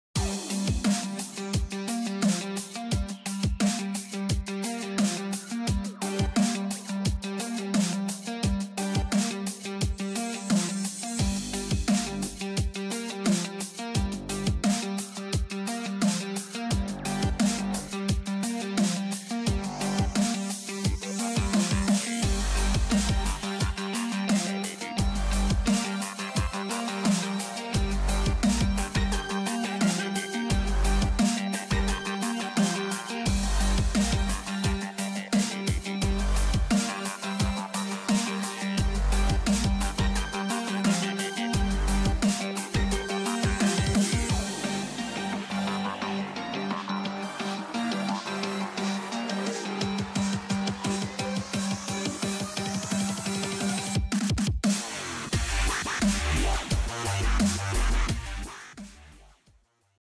Pretemporada 2018 de F1 - Día 6
Resumen de lo ocurrido en el Circuit de Barcelona-Catalunya durante la sexta jornada de test de Fórmula 1.